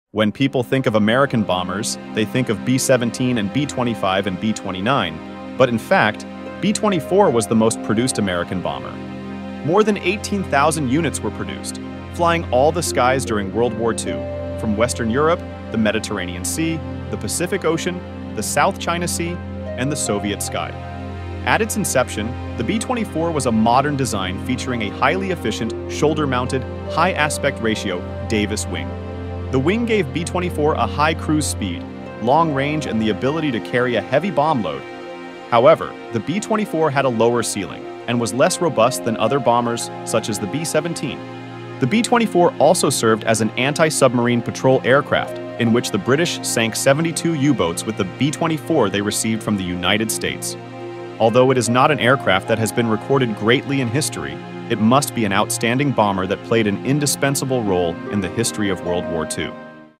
America's Most Produced Bomber : B-24 Liberator
When people think of American bombers, they think of B-17 and B-25 and B-29, but in fact, B-24 was the most produced American bomber.

More than 18,000 units were produced, flying all the skies during World War II, from Western Europe, the Mediterranean Sea, the Pacific Ocean, the South China Sea, and the Soviet sky.

At its inception, the B-24 was a modern design featuring a highly efficient shoulder-mounted, high aspect ratio Davis wing.

The wing gave B-24 a high cruise speed, long range and the ability to carry a heavy bomb load.

However, the B-24 had a lower ceiling and was less robust than other bombers such as the B-17.

The B-24 also served as an anti-submarine patrol aircraft, in which the British sank 72 U-boats with the B-24 they received from the United States.

Although it is not an aircraft that has been recorded greatly in history, it must be an outstanding bomber that played an indispensable role in the history of World War II.